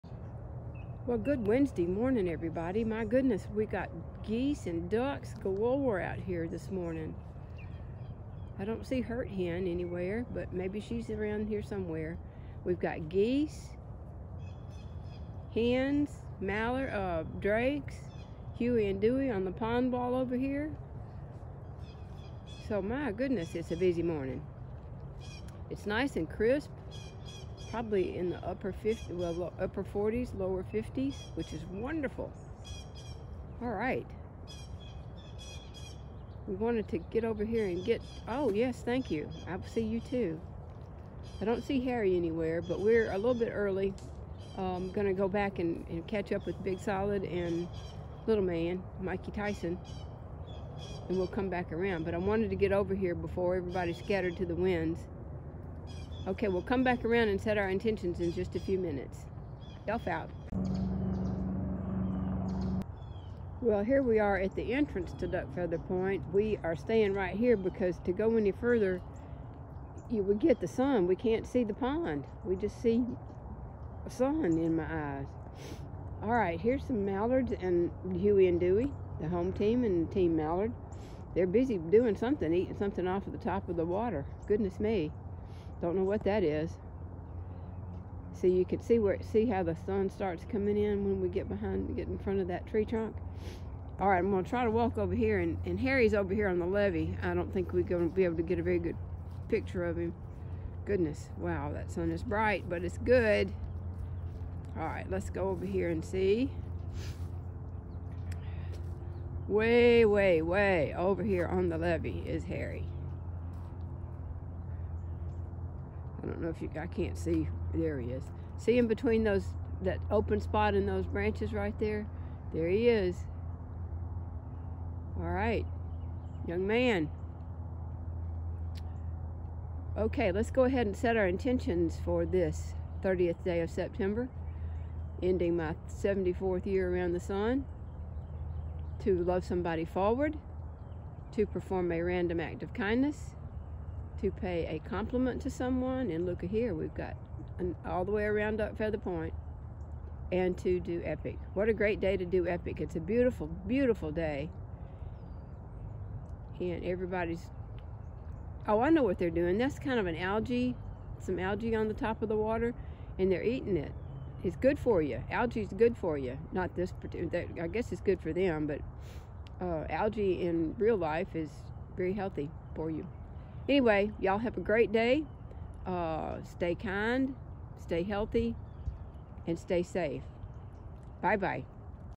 "Saying Goodbye to September AND The Elf Looks at 74"©
Another gorgeous Fall morning in Pondlandia! Lots of busy-ness on the South Bank as the Geese, Team Mallard, and the Home Team had just gotten a big ole cup ‘Full ‘O Corn’ from the Keep of said Corn. Food coupled with the cool weather makes our feathered friends happy and active. Some chasing going on but by and large a hospitable group.
We accidentally flushed Harry out of his spot in the northwest curve of the pond, where we found him yesterday but we were able to get a few seconds of his flight away from us. He landed on the levee where we caught him a bit later in the last part of the video.
The Pond has a film of green algae on it and the ducks were seemingly enjoying slurping it down. We do know that algae (not THIS algae) has some health benefits such as Omega-3 Fatty Acids and other essential Amino Acids (google it). So maybe they are aware of what their nutritional needs are!!
As we begin our 75th trip around the sun, we are hopeful that y’all continue to enjoy The Chronicles of Pondlandia and also continue to DO EPIC!! Every day…stay safe, stay healthy, and always always BE KIND! Elf Out!
Oh...and PS. Big Solid has yet to break 80 on the golf course but he's going to keep trying!